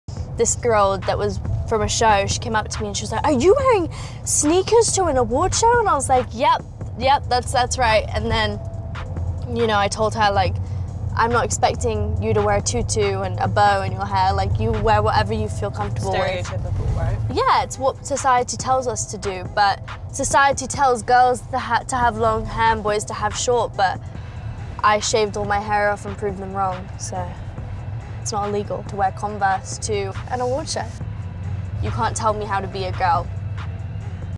All Her Stories Are True- Millie Bobbie Brown
"Tú no puedes decirme cómo ser una mujer"- Millie Bobbie Brown. Descubre qué hizo que Millie Bobbie Brown respondiera así en una alfombra roja.